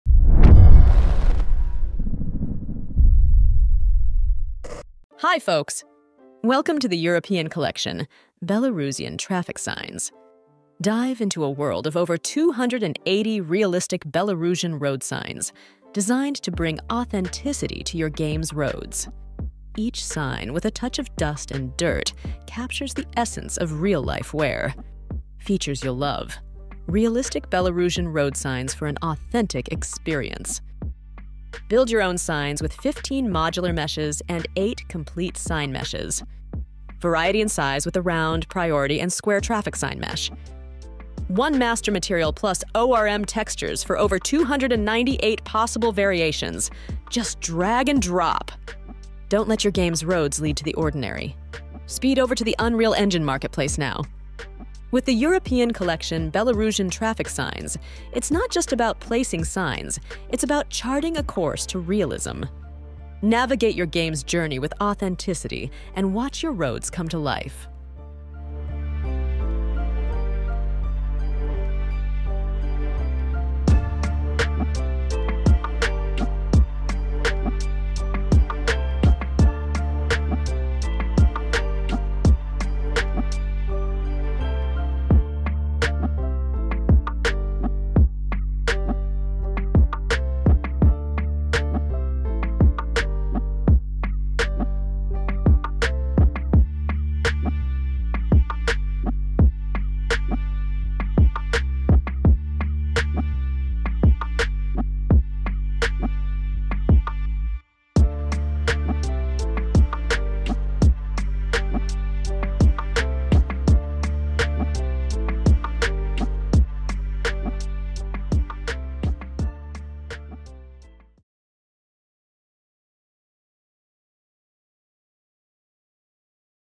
Game Ready Belarusian Road Signs — Traffic Signs for Unreal Engine
Explore the European Collection: Belarusian Traffic Signs Bundle for Unreal Engine! Boasting over 280 realistic signs, this pack enhances your game's authenticity effortlessly. Modular and complete meshes offer a realm of customization. Drive your game's realism to a new level! 🚦🎮

FEATURES:
🚀 Easy DragN Drop System
🛠️ MODULAR Sign Meshes for your Game
🔄 More than 280 Sign VARIATIONS
⏰ Save precious TIME and enhance flexibility

0:00 - Belarusian Traffic Signs for Unreal Engine
2:00 - Outro